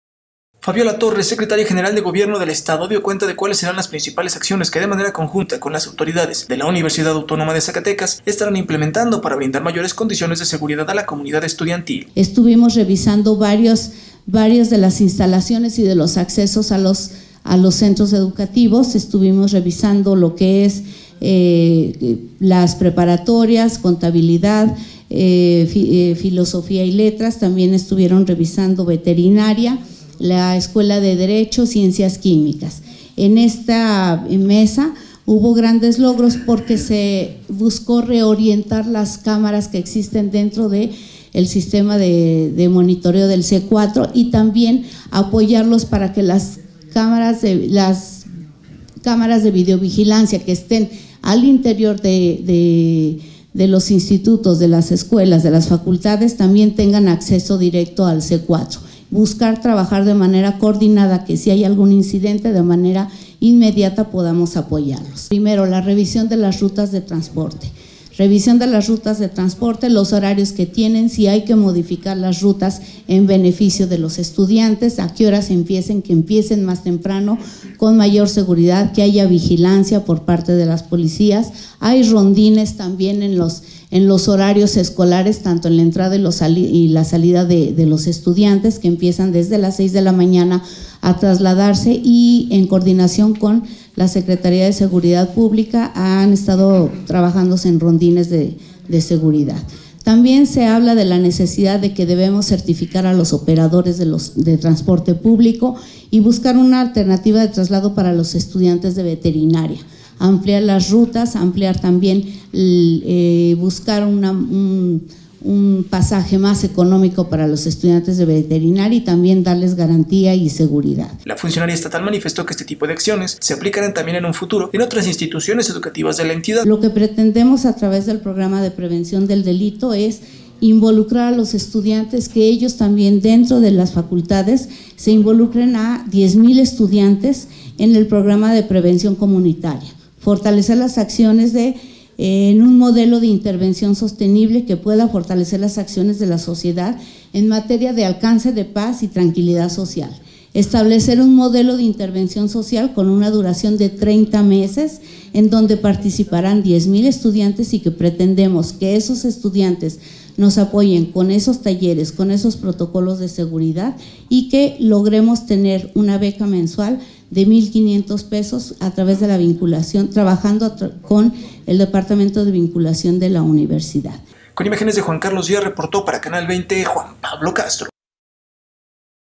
Anuncian acciones para elevar condiciones de seguridad a comunidad escolar de la UAZ
Fabiola Torres secretaría general de Gobierno del Estado dio cuenta de cuáles serán las principales acciones que de manera conjunta con las autoridades de la Universidad Autónoma de Zacatecas estarán implementando para brindar mayores condiciones de seguridad a la comunidad estudiantil.
La función estatal manifestó que este tipo de acciones se aplicarán en un futuro también en otras instituciones educativas de la entidad, sobre todo las relacionadas con la prevención del delito